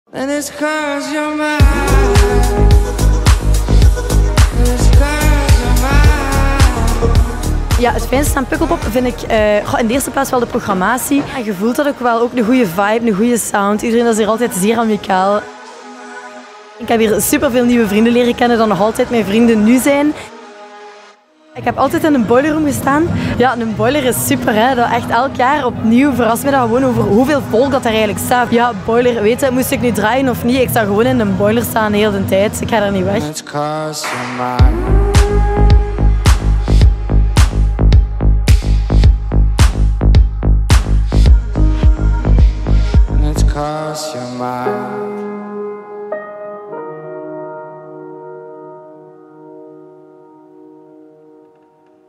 Charlotte de Witte ft. Boiler Room: match made in heaven
"Het fijnste aan Pukkelpop? De programmatie zorgt voor een unieke vibe." Charlotte de Witte weet waaraan ze zich kan verwachten in de Boiler Room. Bevestigd tijdens haar set: de ravissante Gentse deejay weet haar publiek in extase te brengen.